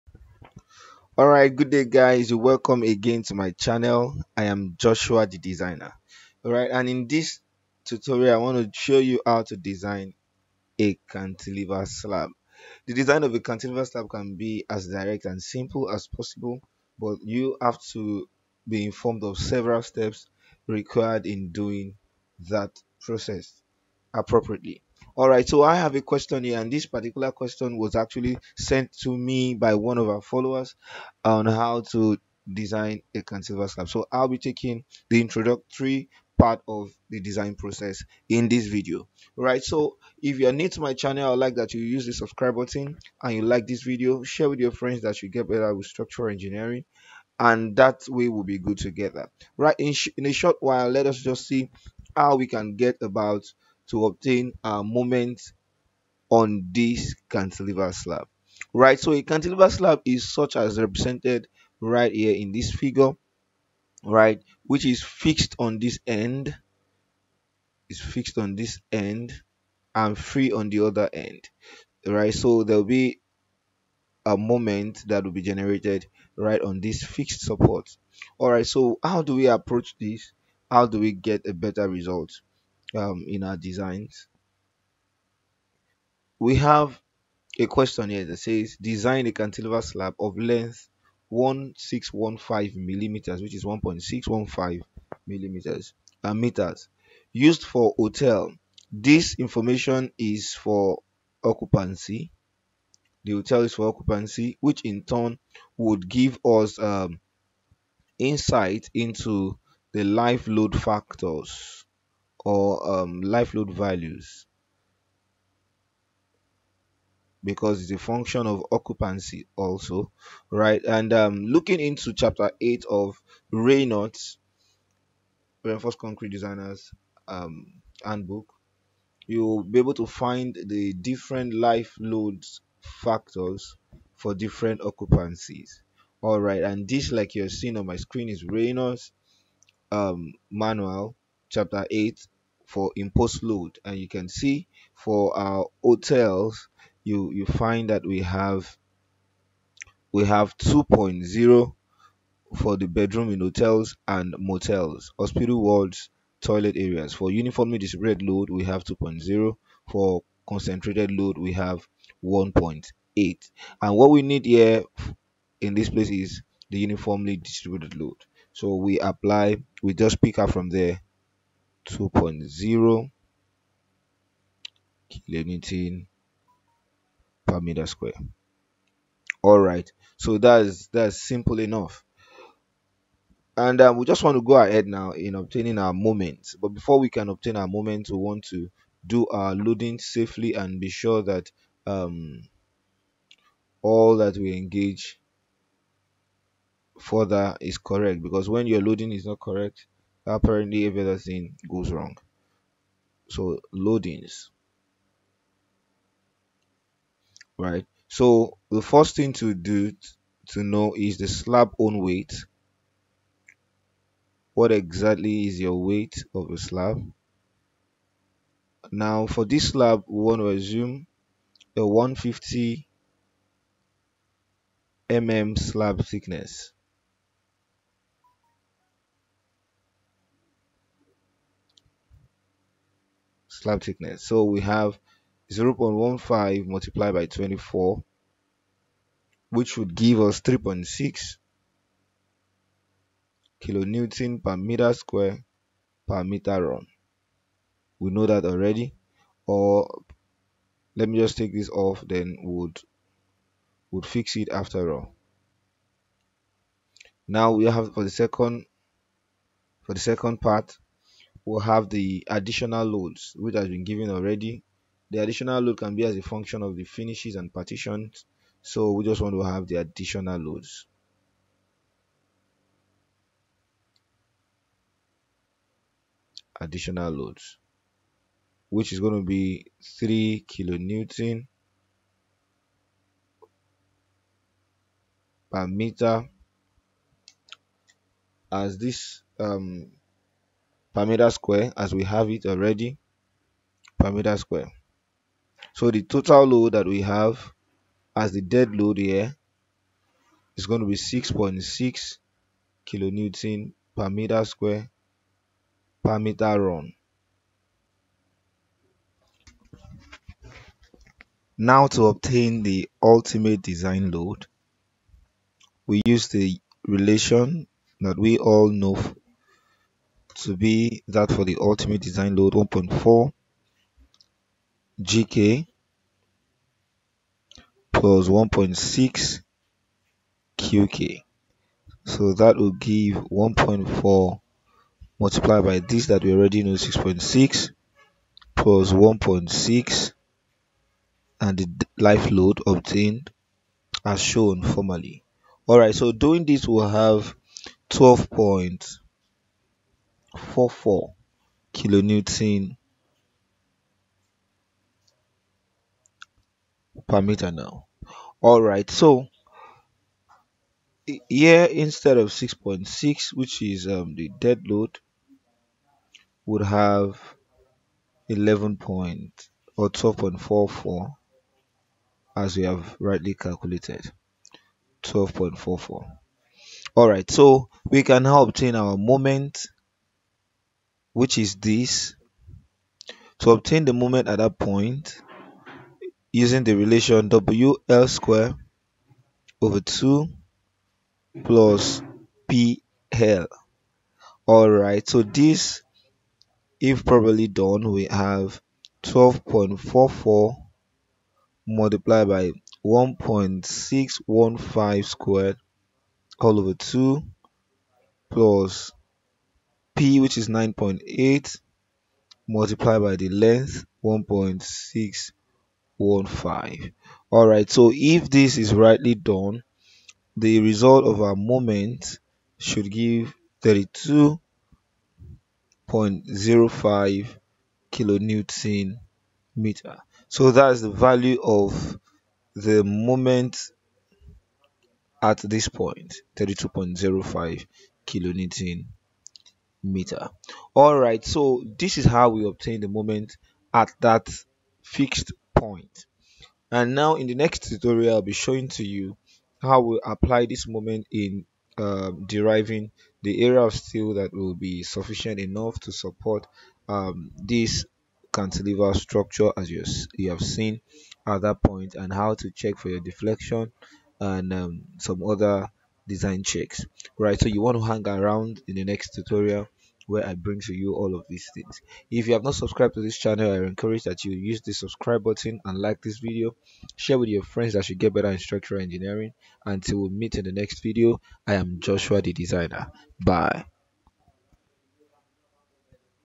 HOW TO DESIGN A CANTILEVER SLAB PT1
Learn the simplest approach to design a cantilever slab in this video